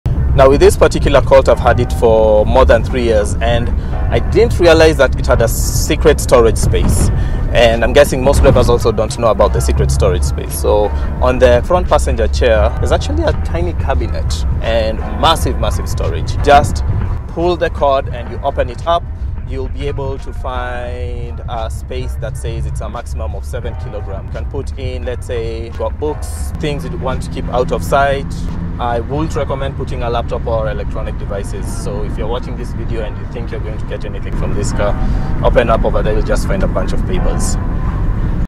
Secret storage in the 2005 Mitsubishi Colt
If you're still looking for storage in the 2005 Mitsubishi Colt 2005 and you have stock seats and no seat cover, look for the pull tab.